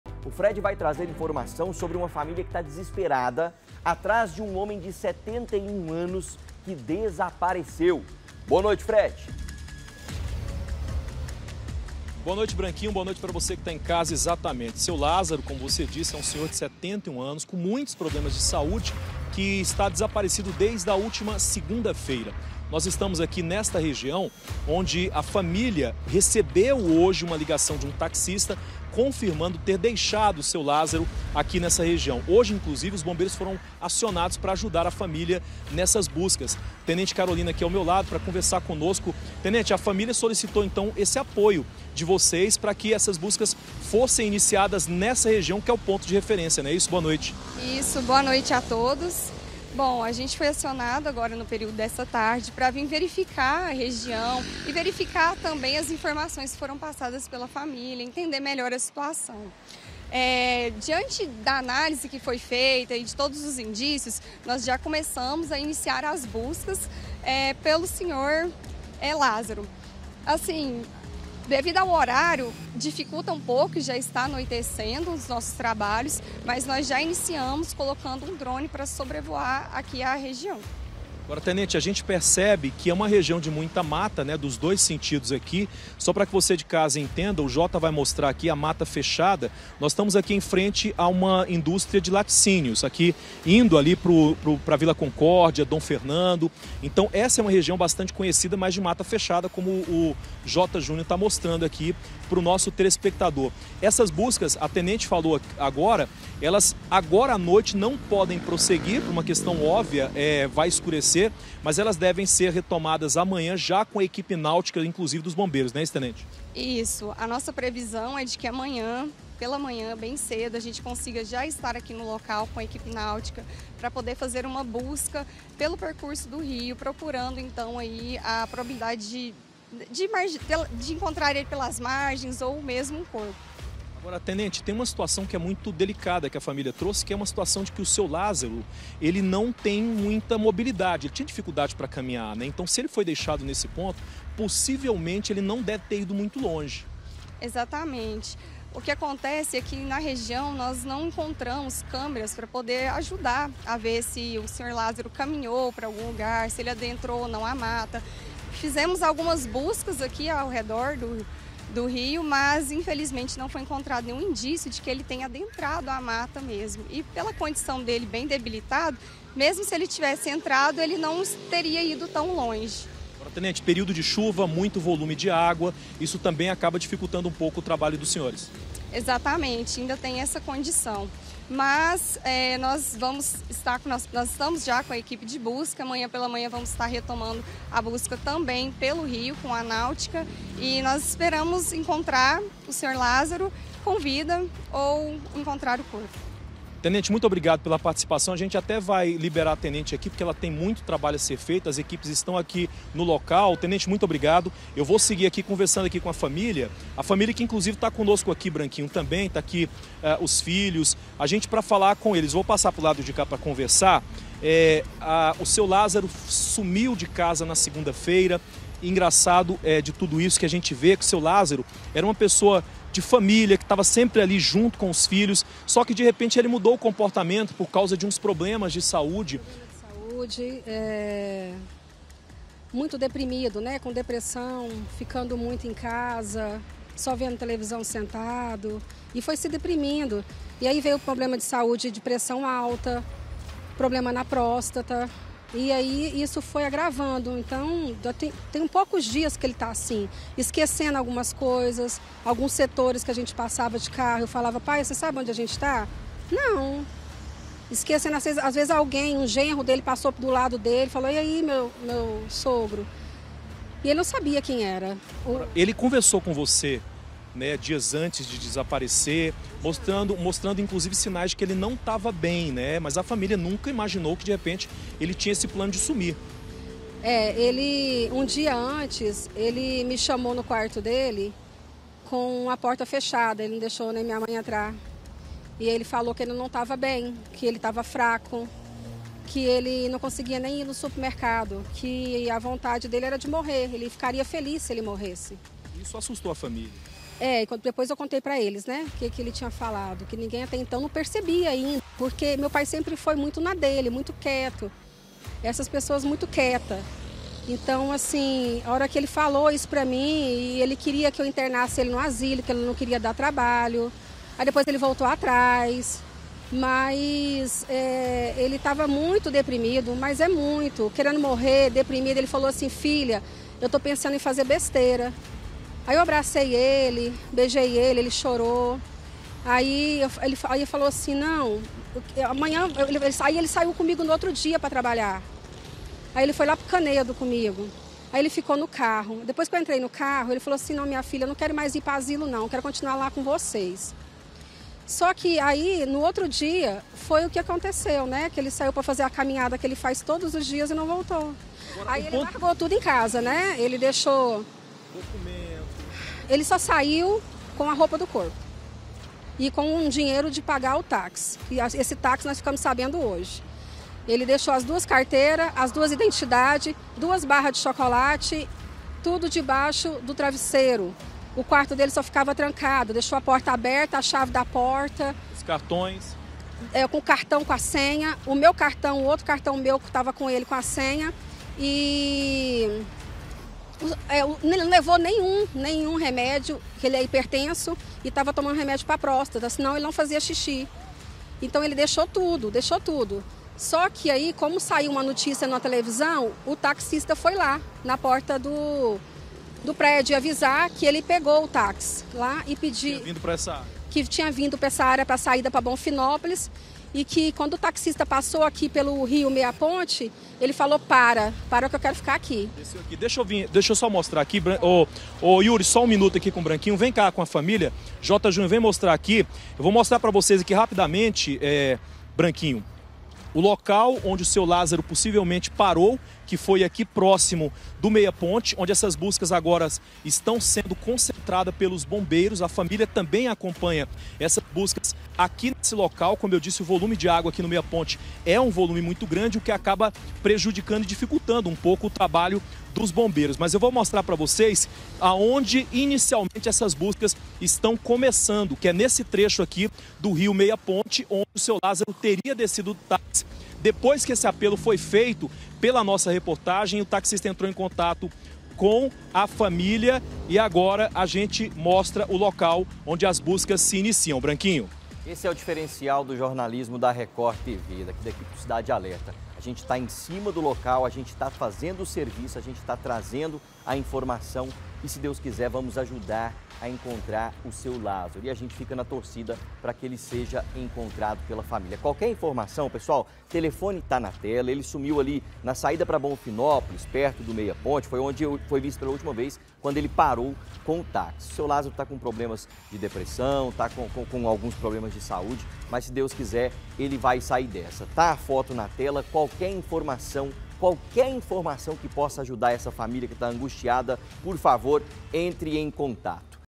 DESAPARECIDO: BOMBEIROS FAZEM BUSCAS PELO LÁZARO
UMA FAMÍLIA ESTÁ DESESPERADA ATRÁS DE UM HOMEM DE 71 ANOS QUE DESAPARECEU HÁ DOIS DIAS. INFORMAÇÕES: (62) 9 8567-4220 - LILIAN DOS SANTOS